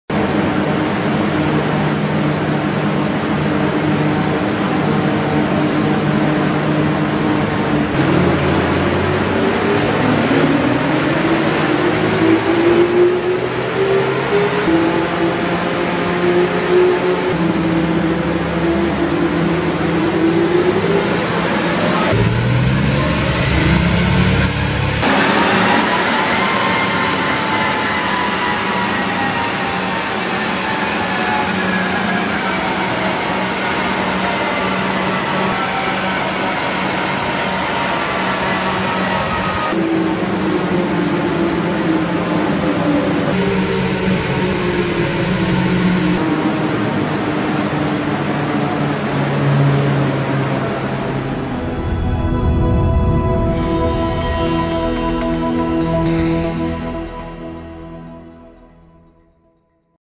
Chiptuning Bmw F31 335xD F on dyno +70bhp & +60nm @ auto-chippen.nl Tilburg
Big improvement for this 335XDrive